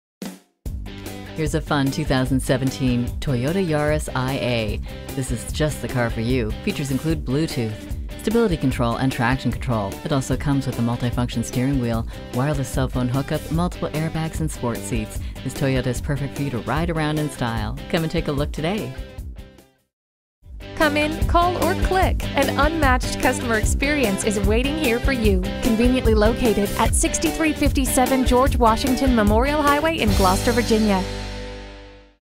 New 2017 Toyota Yaris iA Gloucester, VA #7284
Year: 2017
Make: Toyota
Model: Yaris iA
Trim: 4D SDN
Engine: 1.5L 4 cyls
Transmission: Automatic 6-Speed
Color: Frost
Stock #: 7284
VIN: 3MYDLBYV2HY146987

Gloucester Toyota offers a full selection of service areas including:
Address:
6357 George Washington Memorial HWY